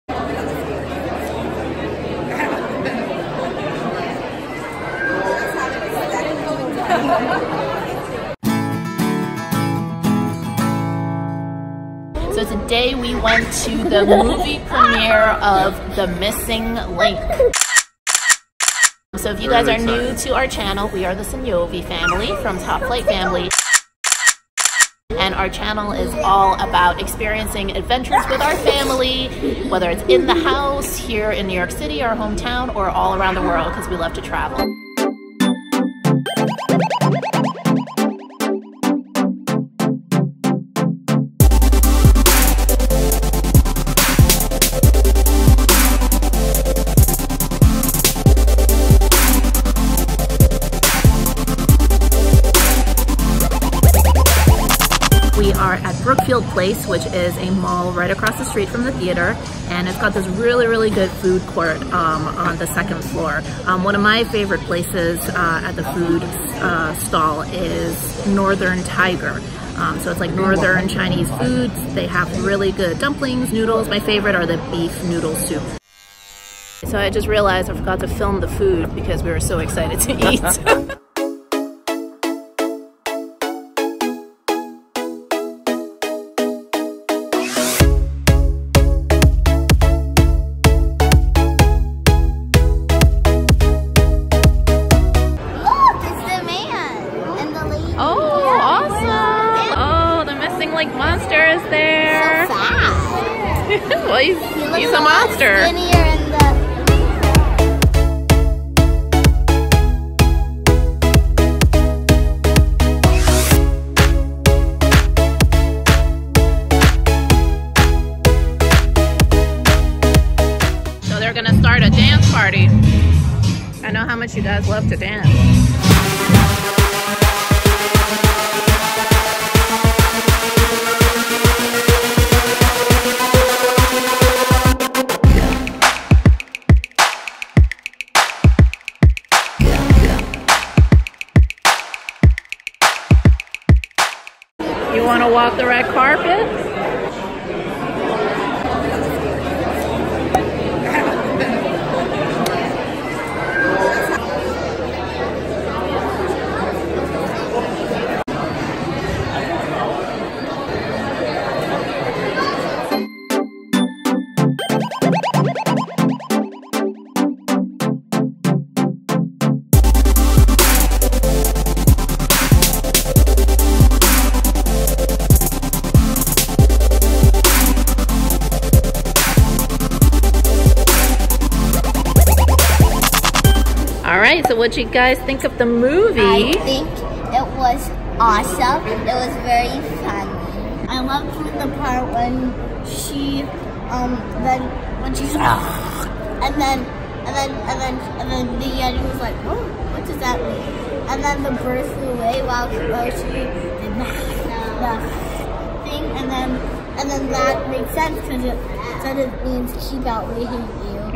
MOVIE PREMIERE FOR THE MISSING LINK MOVIE - Top Flight Family - Luxury Family Travel
Have you ever dreamt of walking the red carpet at a fancy Hollywood movie premiere? We just had this fun experience! The team behind The Missing Link movie invited us to the red carpet premiere in New York City. Not only did we get to try walking the red carpet, but we got to see the movie's stars Hugh Jackman, Zoe Saldana and Zach Galifianakis!

2) ✅  FREE GUIDE: "The 10 Travel Products Every Family Should Own"

5) 🎥 WATCH THESE RECOMMENDED VIDEOS